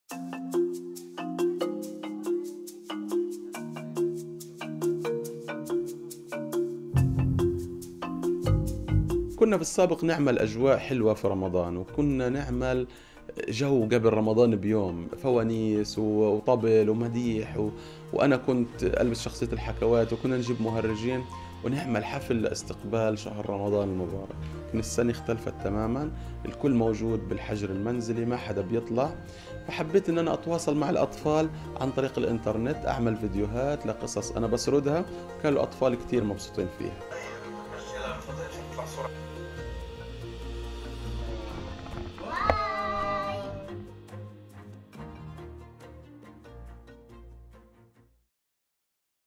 A storyteller in Gaza livestreams for children during Ramadan
Gazan storyteller Fida al Lidawi has been entertaining children from Nusseirat refugee camp with storytelling sessions during the month of Ramadan for the last 18 years. This year he goes online due to the coronavirus restrictions